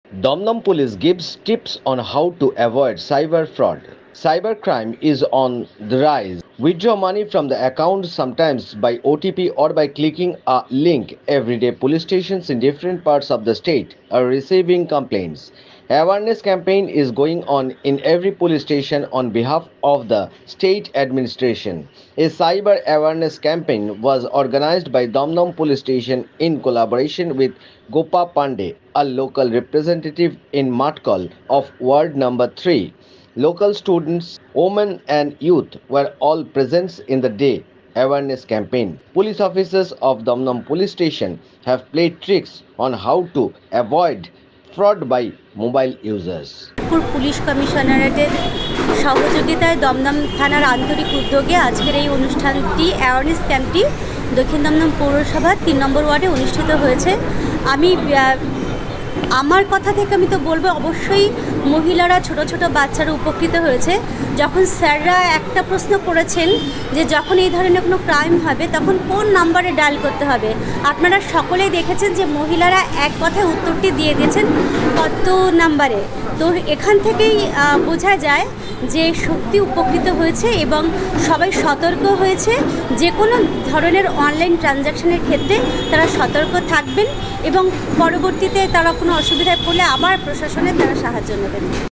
cyber security campaign at DumDum
Cyber security campaign at DumDum was held on Sunday, Gopa Pandey councillor of ward no 3 organised this event.